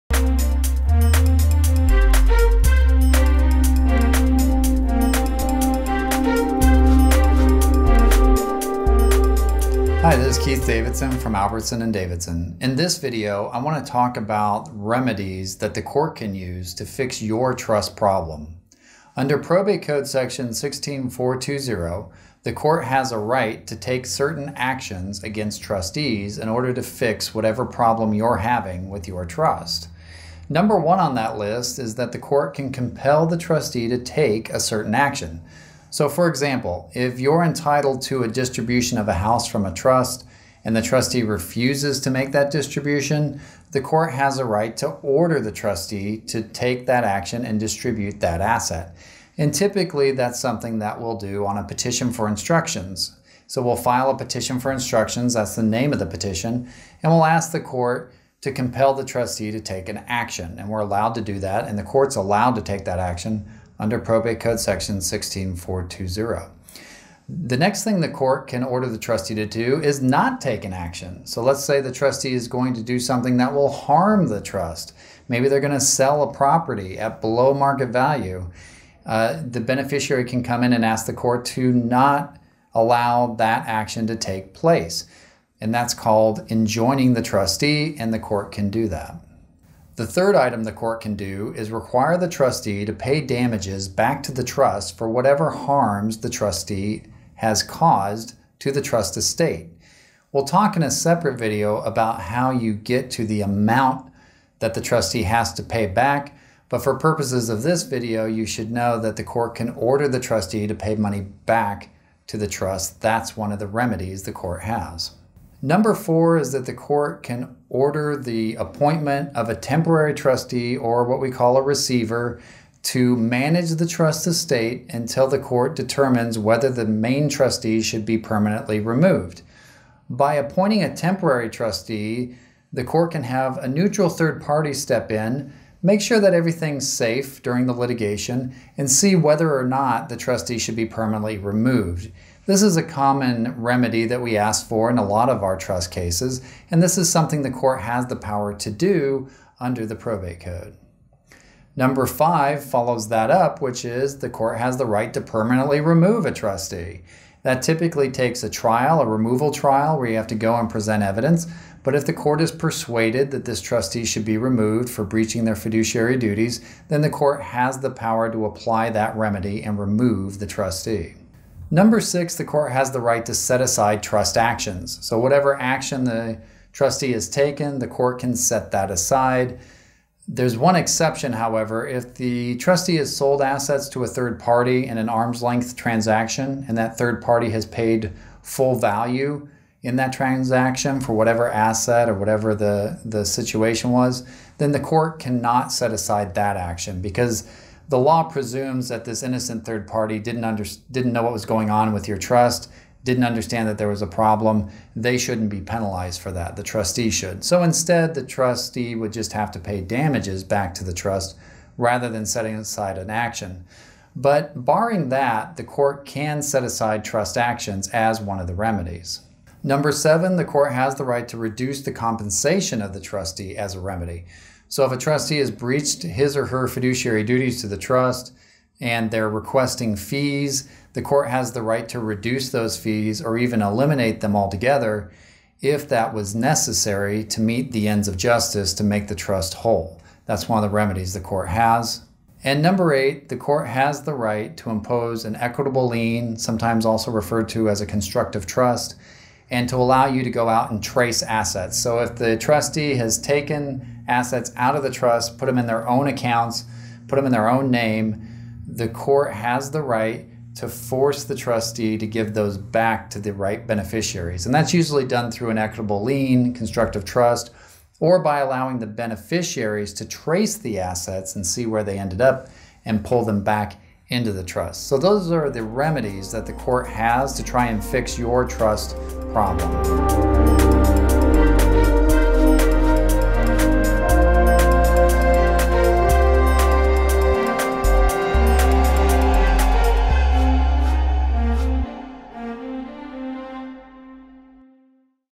What Can a Court Do To Repair Your Trust?
If a Trustee commits a breach of Trust, or threatens to do so, California law allows a beneficiary or a co-Trustee to take action. The type of action you can take depends on on the type of breach committed. Attorney Keith Davidson explains the remedies the court can use to fix your trust problems.